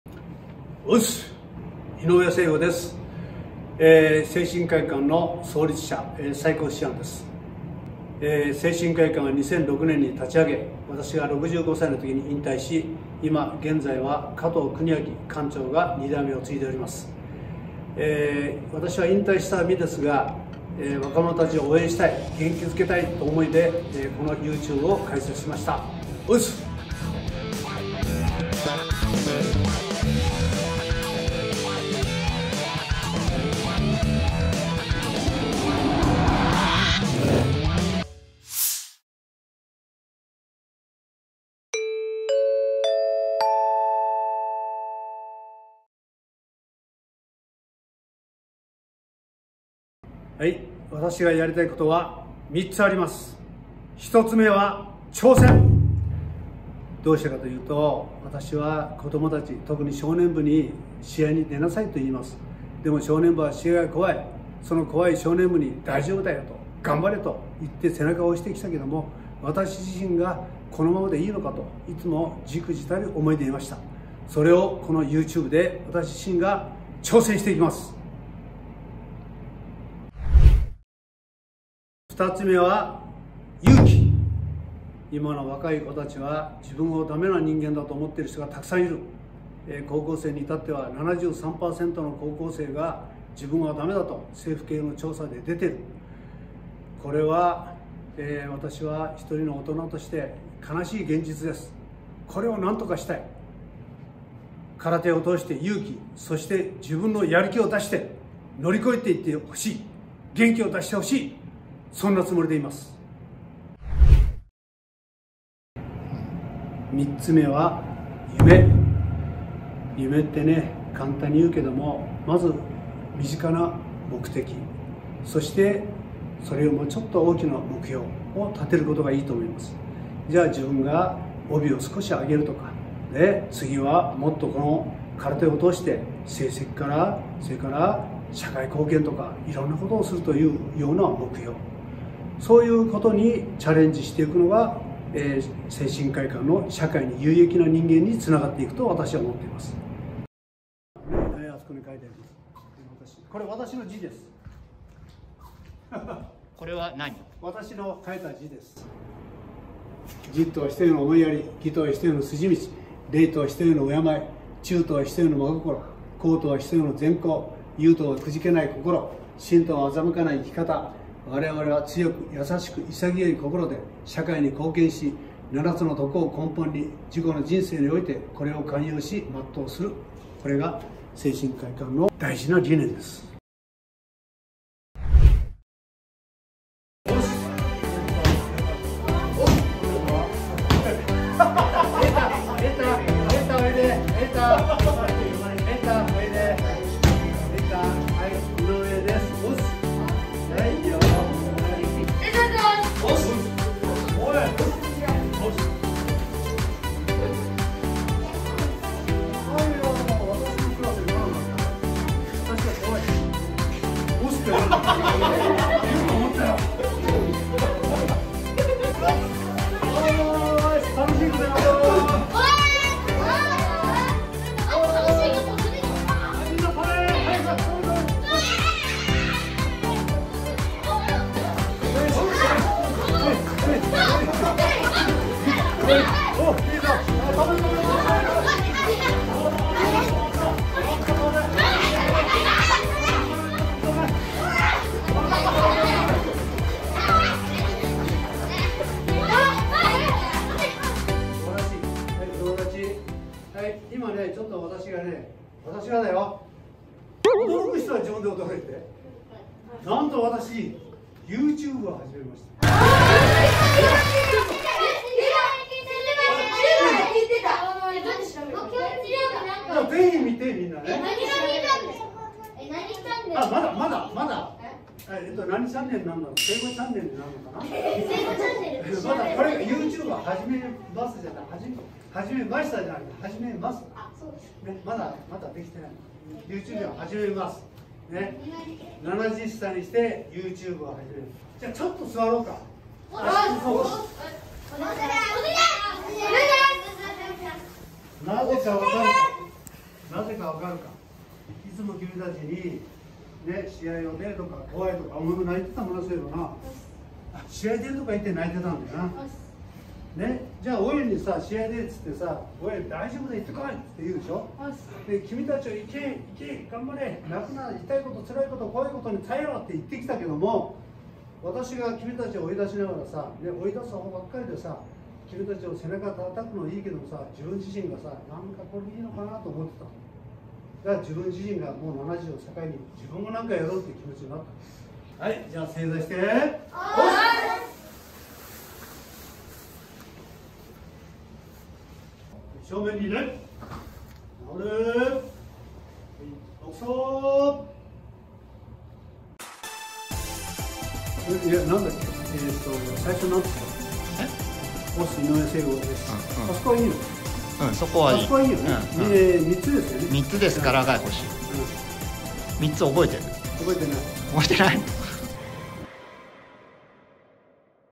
【初投稿】今年７０歳YouTuberデビュー
今年で70歳！
誠真会館の創立者＆最高師範＆元JAC＆作家
道場は全国２３箇所・道場生は１２００人以上
“自己肯定力を高める指導”をモットーに指導。
いじめられっ子→全日本チャンピオン！
クラスで劣等生→高校で進学校！
空手を通じて子どもたちに夢を持たせる！

文京区（本郷）豊島区（落合南長崎）杉並区（西荻窪）で指導